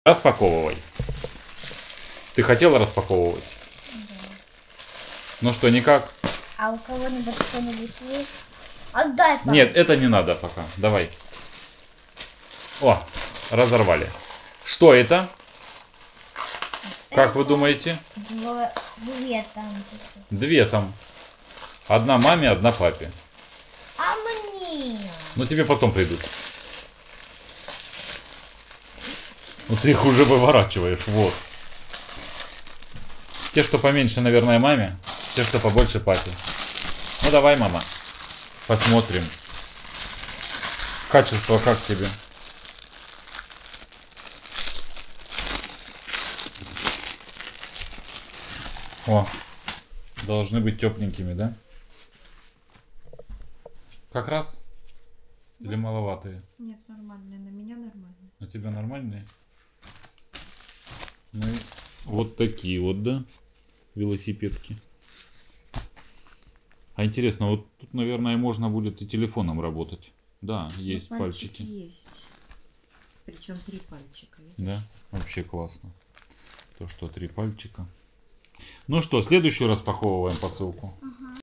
Велосипедные перчатки для телефона
Какие бывают велосипедные перчатки, для чего они нужны и как подобрать перчатки под себя и свое катание.